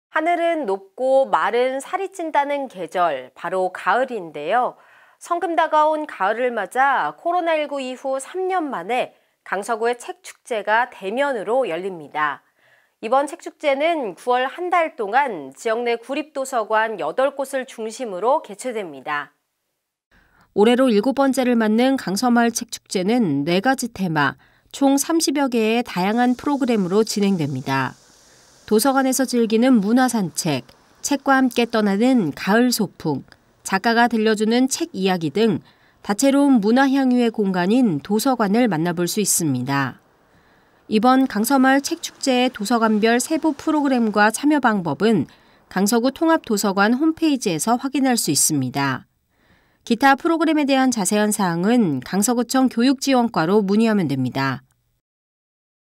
[CC] 9월 한달간, 제7회 강서마을책축제 개최
하늘은 높고 말은 살이 찐다는 계절,
바로 가을인데요.

성큼 다가온 가을을 맞아 코로나19 이후 3년 만에
강서구의 책 축제가 대면으로 열립니다.

이번 책 축제는 9월 한달 동안
지역 내 구립도서관 8곳을 중심으로 개최됩니다.

올해로 일곱 번째를 맞는 강서마을책축제는
4가지 테마, 총 30여개의 다양한 프로그램으로 진행됩니다.

도서관에서 즐기는 문화 산책, 책과 함께 떠나는 가을소풍,
작가가 들려주는 책 이야기 등
다채로운 문화 항유의 공간인 도서관을 만나볼 수 있습니다.

이번 강서마을책축제의 도서관별 세부 프로그램과 참여방법은
강서구 통합도서관 홈페이지에서 확인할 수 있습니다.

기타 프로그램에 대한 자세한 사항은

- 영어, 중국어, 일본어
- 베트남어